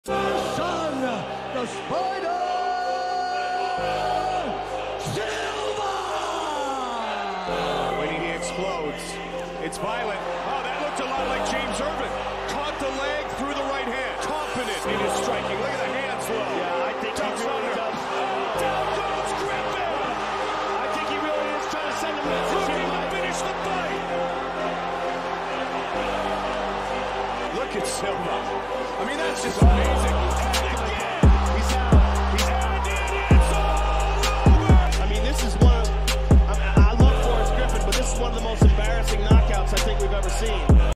Anderson Silva EMBARASSING KNOCKOUT win over Forrest Griffin
Anderson Silva vs Forrest Griffin highlights

we will act upon as quickly as possible